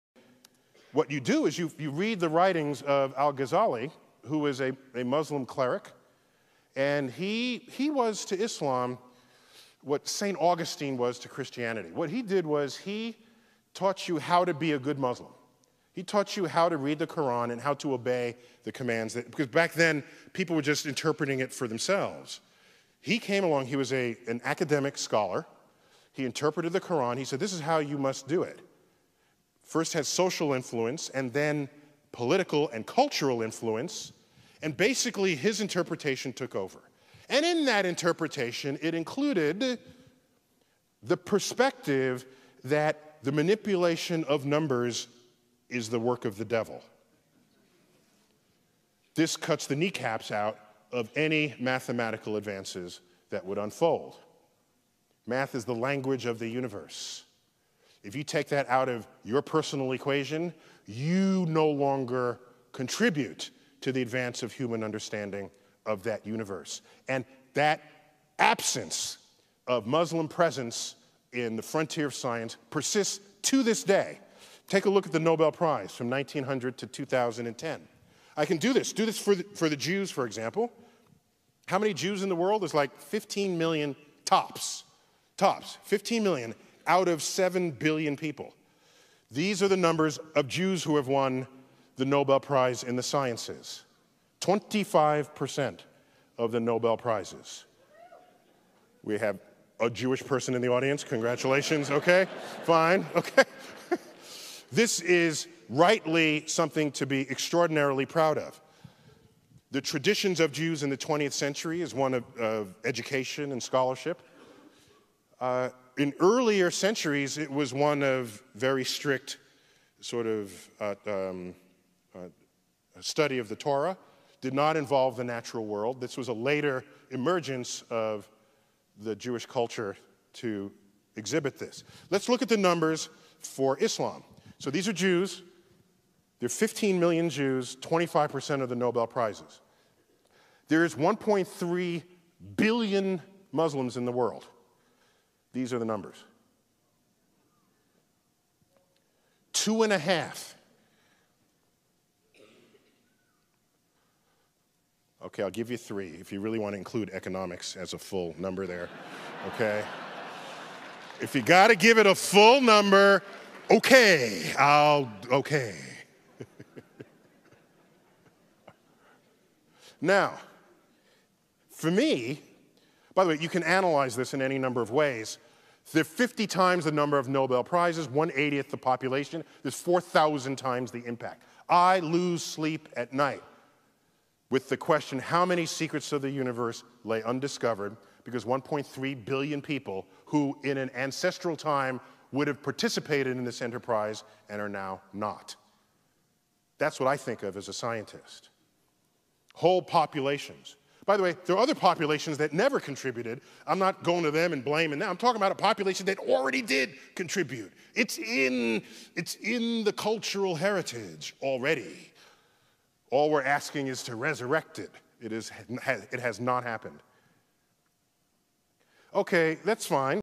Neil DeGrasse Tyson on the Intellectual Collapse of Islam
Neil DeGrasse Tyson explains when Islam, formerly a glorious religion of wisdom and enlightenment, collapsed and opted for magic and primitive beliefs rather than the continued advancement of science, all due to the demonization of mathematics and the emerging sciences of the time.